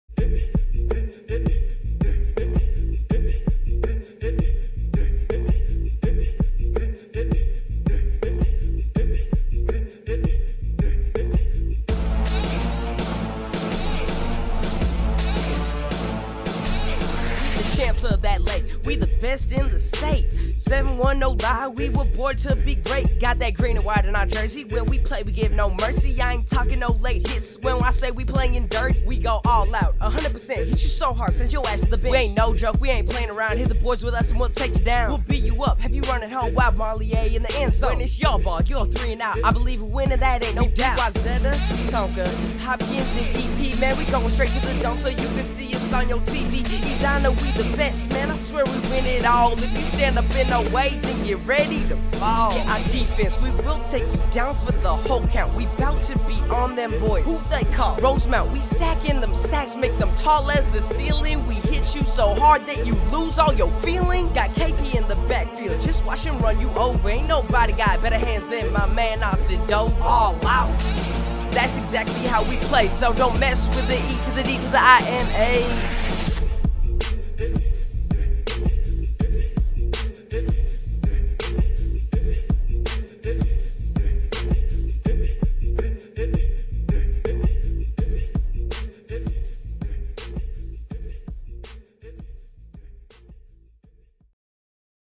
JC-Edina Football (part2)
Made the first one back in 9th grade, now we going to state so I figured we need a new one.